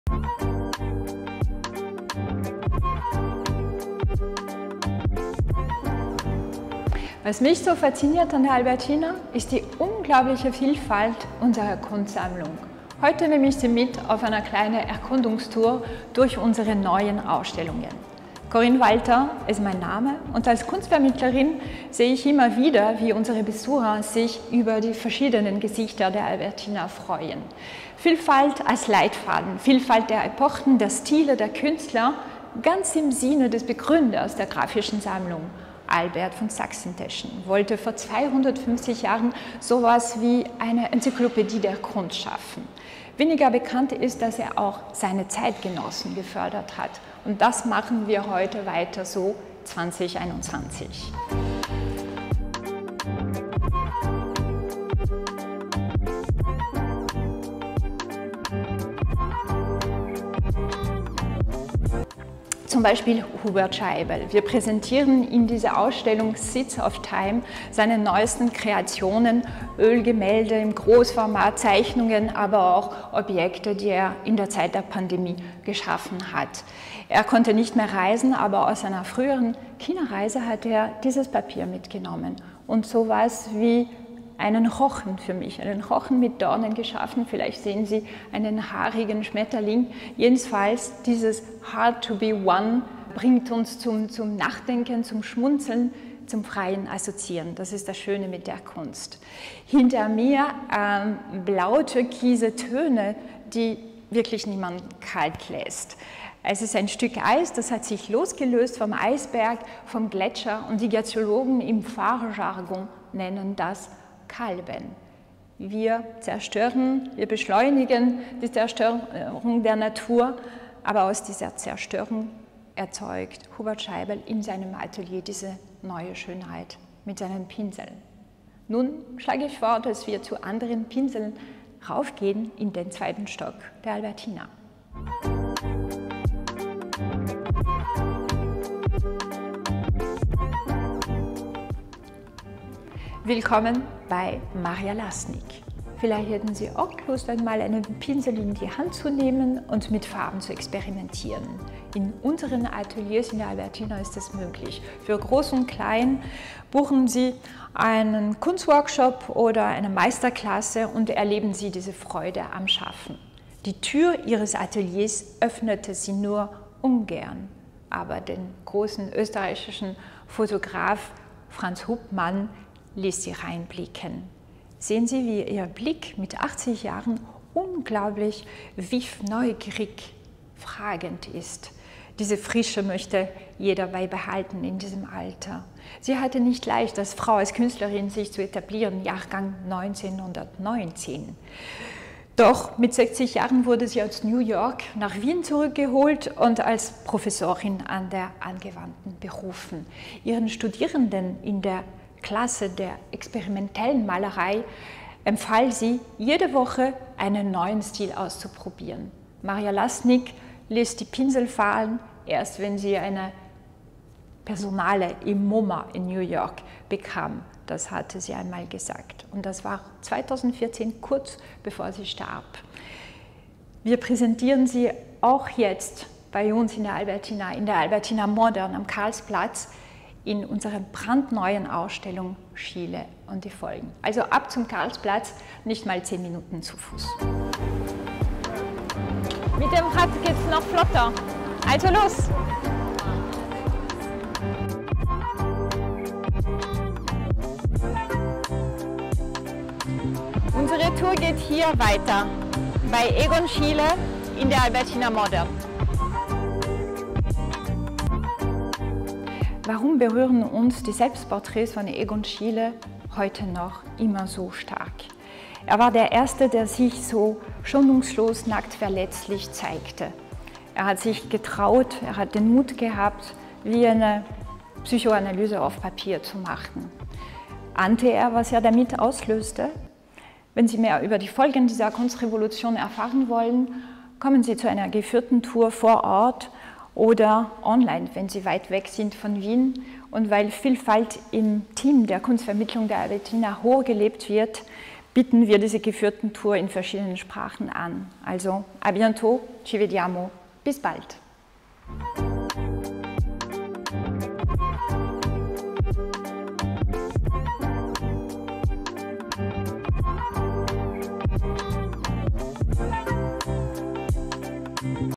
Kommt mit in die Albertina und die Albertina Modern
600 Jahre Kunstgeschichte unter zwei Dächern!
Das Ausstellungsprogramm der ALBERTINA ist vielfältig - genauso wie die Kunst in der Sammlung. Kunstvermittlerin Corinne Walter gibt einen Einblick in aktuelle Ausstellungen und die Kunstvermittlungsprogramme des Museums. Seid ihr bereit?